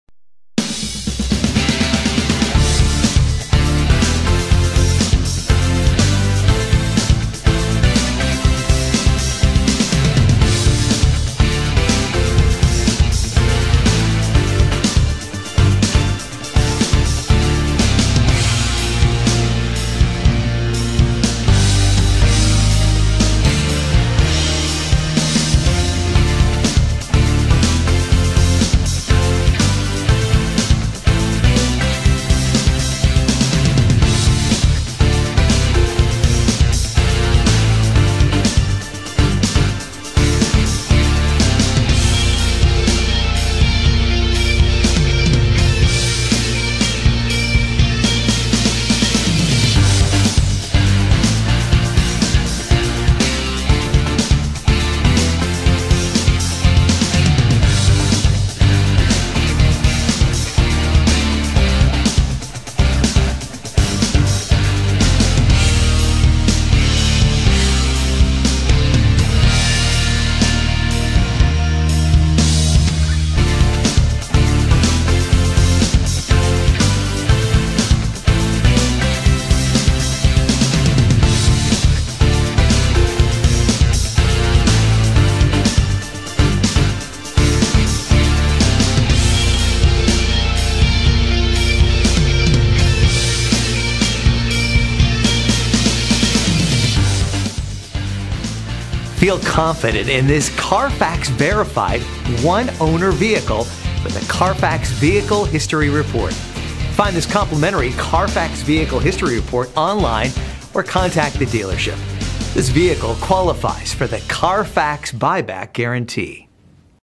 Used 2018 Jeep Grand Cherokee Limited, Willow Grove, PA R21065B
Piazza Jaguar Land Rover Willow Grove

CARFAX One-Owner. Clean CARFAX. Velvet Red Pearlcoat 2018 Jeep Grand Cherokee Limited 4WD 8-Speed Automatic 3.6L V6 24V VVT Fully Serviced, Passed Safety Inspection, Warranty, Quick Order Package 2BH. Priced below KBB Fair Purchase Price!Odometer is 8779 miles below market average!Awards:* 2018 KBB.com 10 Most Awarded Brands

[RLVID:4358:afb9314ebe31f7bc:16bd027bd83fa87e:1]